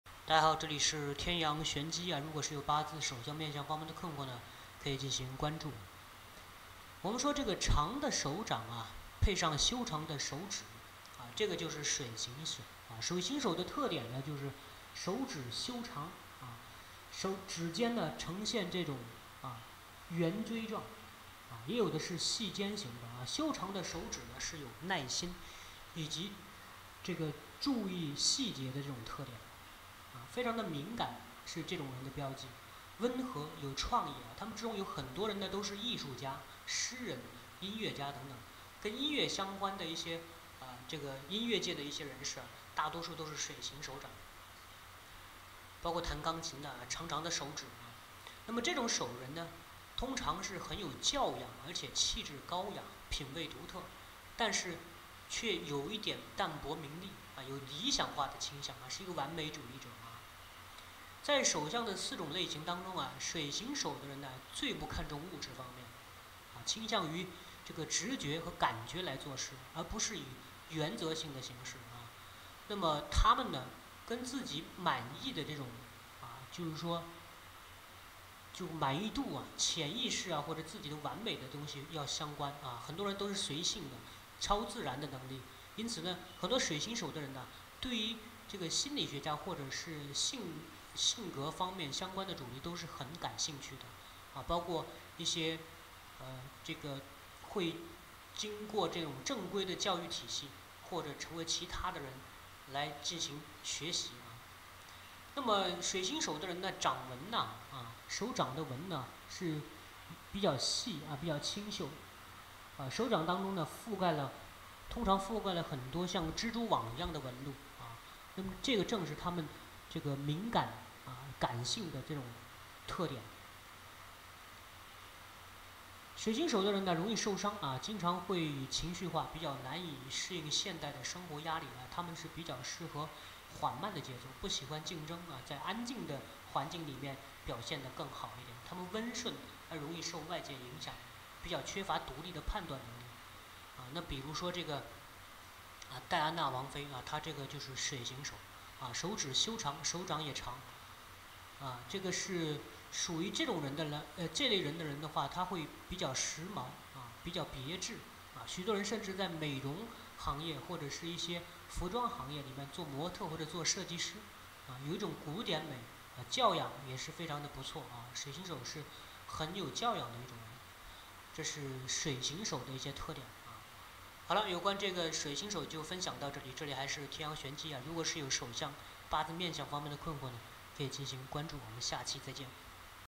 手相教学系列之水型手：温柔敏感，天生的艺术家！戴安娜王妃就是！
师傅微信；tianyangxuanji